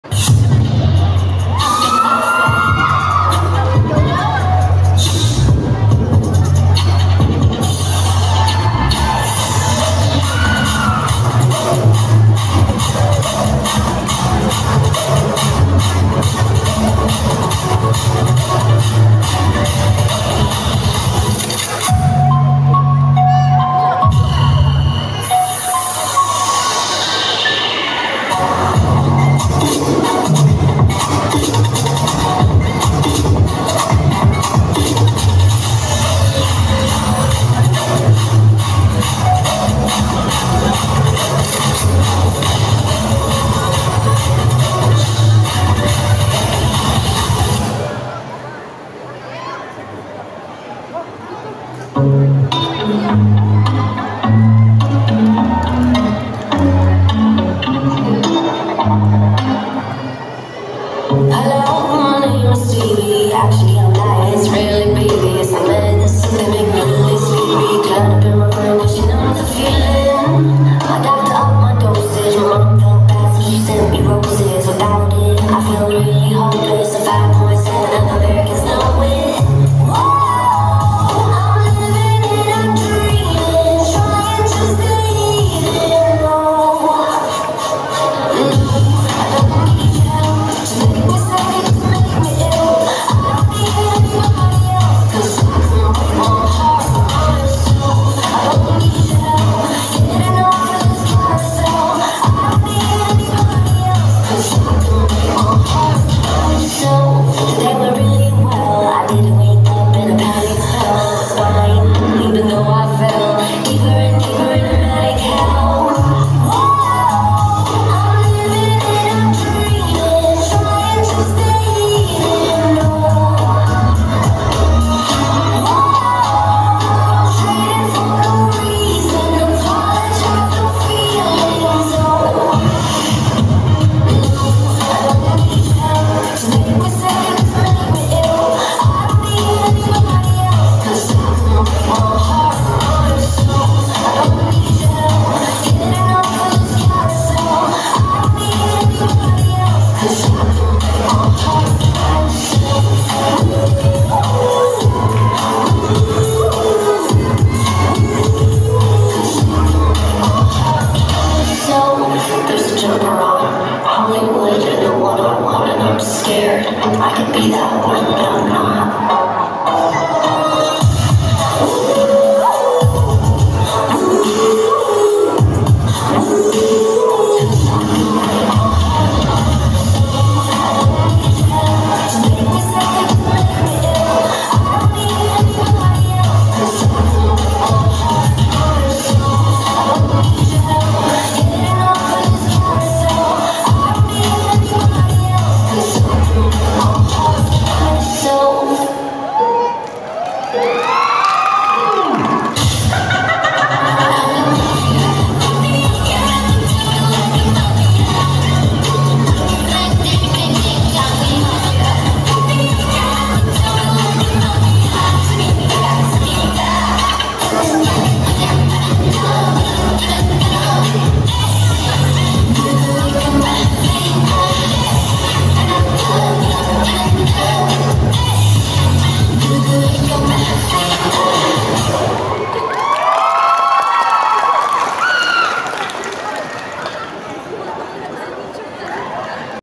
Rizura Dance Cover ITZY at K-Show Festival 2023 Mall Ciputra Tangerang 230923
Group : Rizura
Cover : ITZY
Event : KShow Festival 2023 by Crown Entertainment